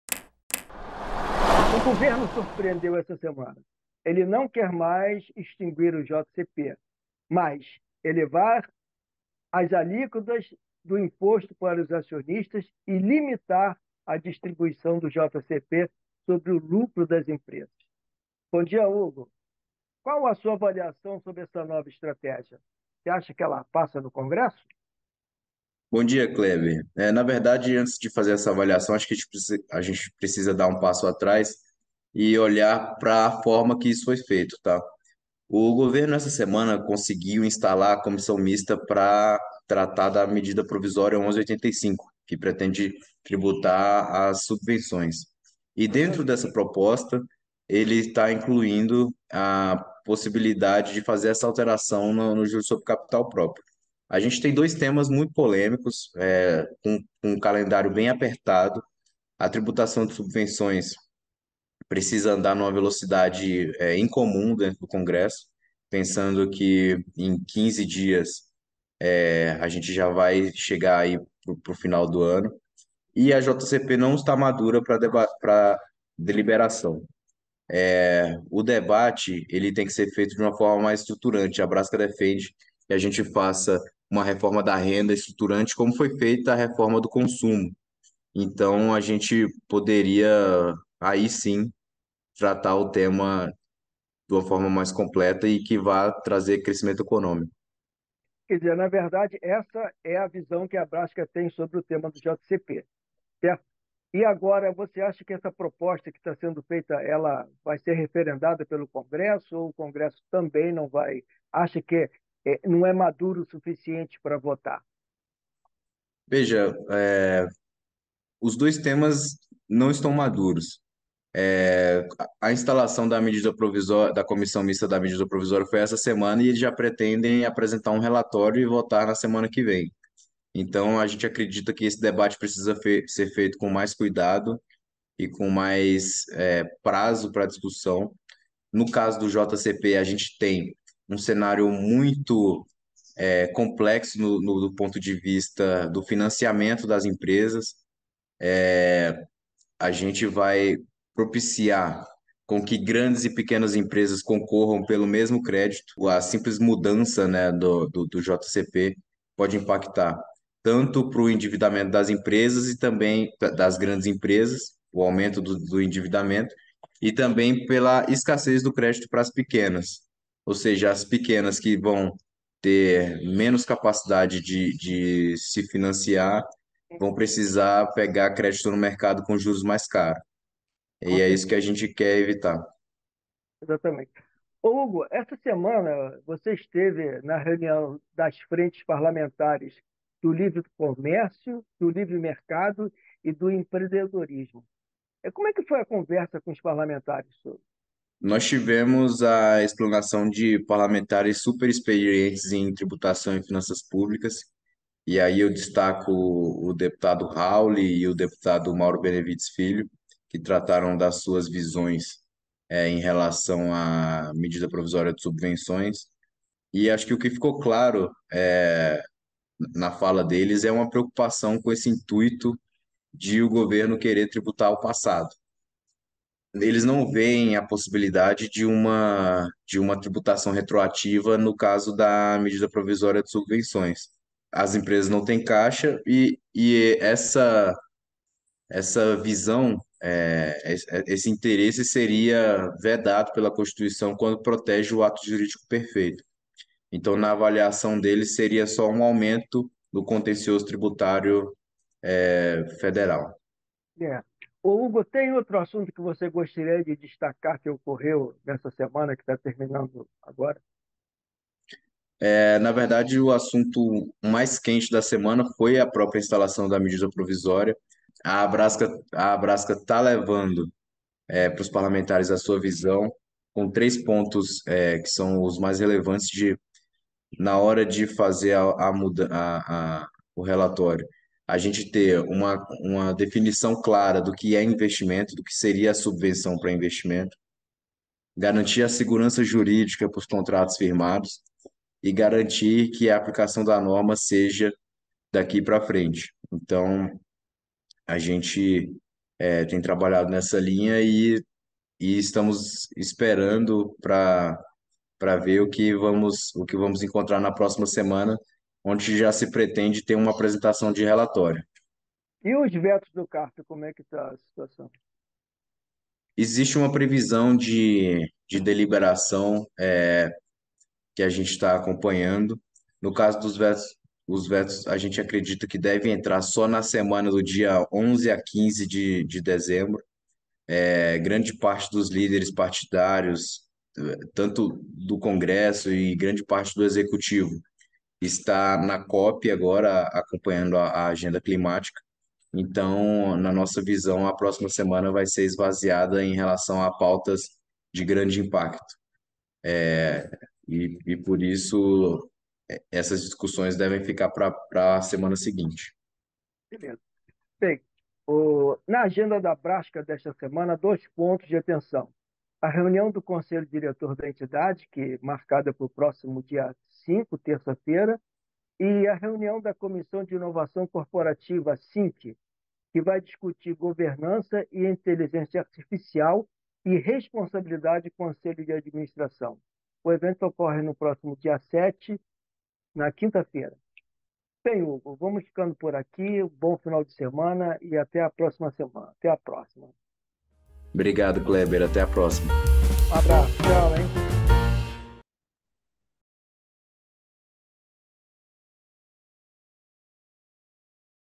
Semana em resumo / 27 de novembro a 1º de dezembro de 2023
Balanço da semana em Brasília e próximos eventos da Abrasca.
Em pauta, vários temas nesta semana: limitação da distribuição de JCP e comissão mista da MP 1.185.
Hugo Leal comenta sobre a reunião com os deputados Luiz Carlos Hauly e Mauro Benevides que teve como pauta a MP das subvenções.
Com Cleber Cabral, jornalista e editor do Sia & Cia e Hugo Leal, gerente de RIG da Abrasca.